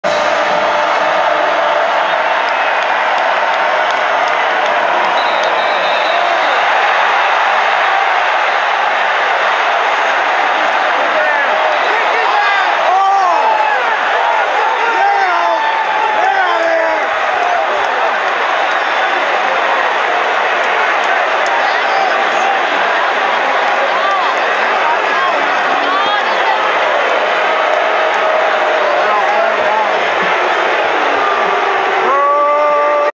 Fight after OSU TD with Oregon at Autzen Stadium
A wild ending to a good play by the beavs. #0 Tre'Shaun Harrison was kicked out for OSU. Offsetting penalties on both teams.